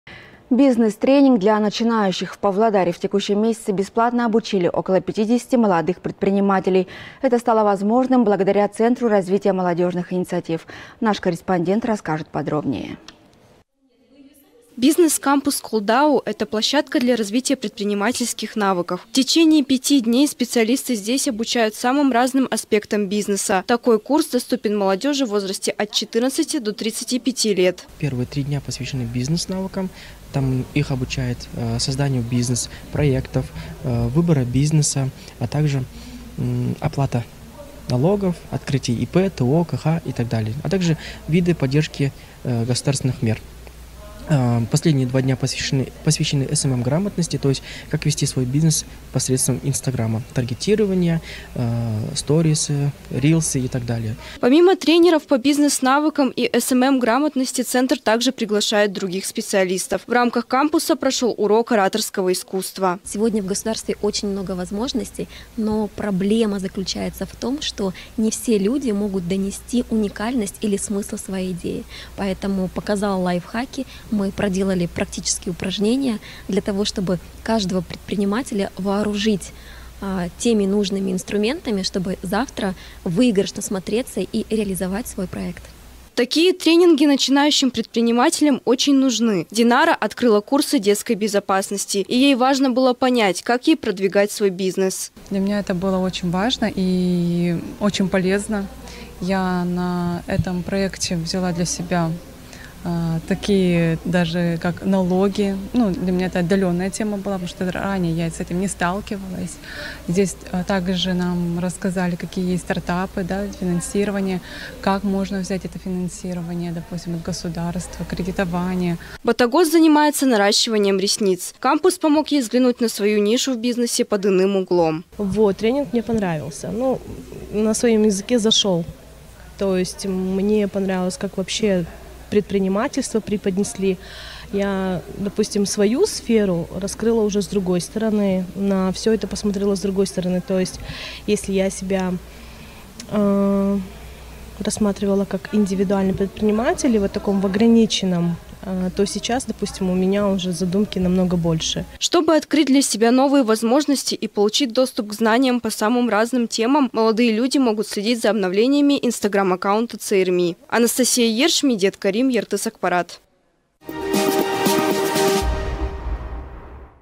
Бизнес-кампус «Qoldau»
Бизнес-тренинги для начинающих. В Павлодаре в текущем месяце бесплатно обучили около 50 молодых предпринимателей. Это стало возможно благодаря Центру развития молодежных инициатив.